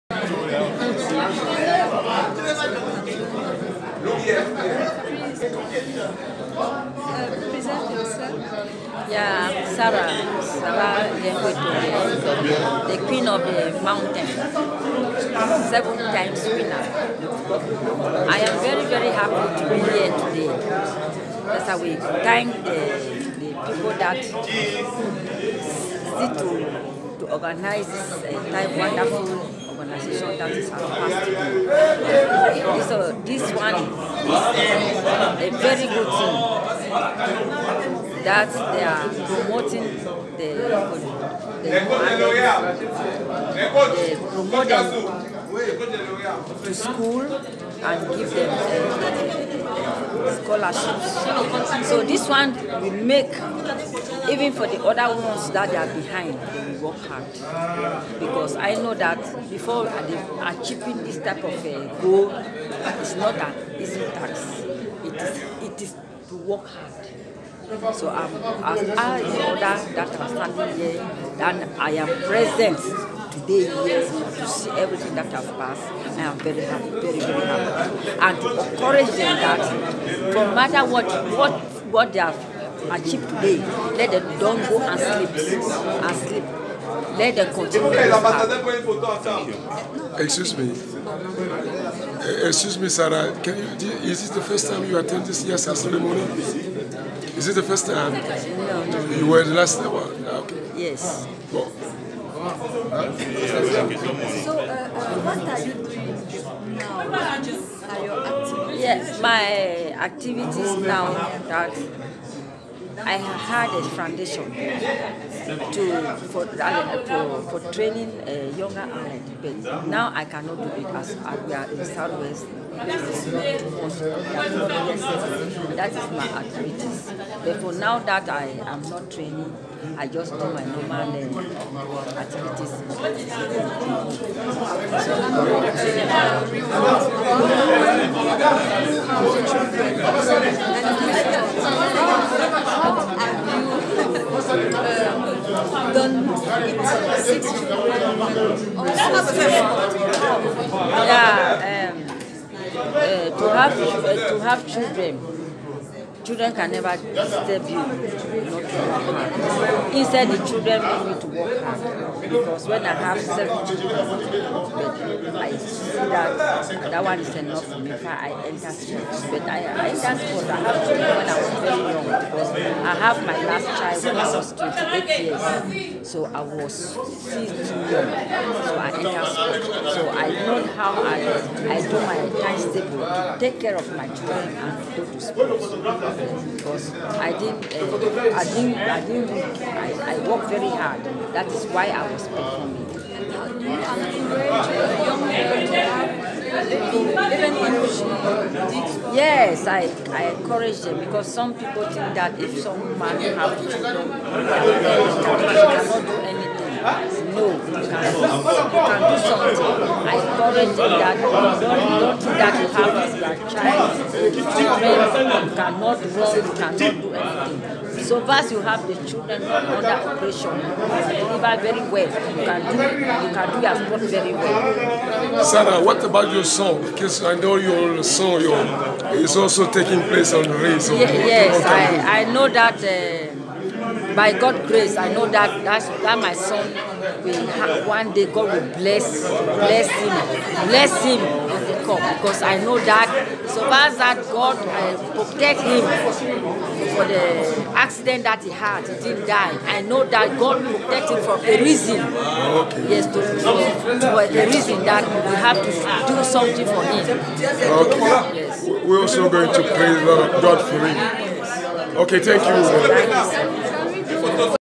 Sarah Etongue thé Queen of the Mountain, invitée à YASSA
YASSA AWARDS 2019 : 2ème Édition
YouTube Academy & Sports Scholarship Assistance.
Cérémonie de Remise de Bourses Académiques de Sports-Etudes.
Sarah Etongue thé Queen of the Mountain, invitée d'honneur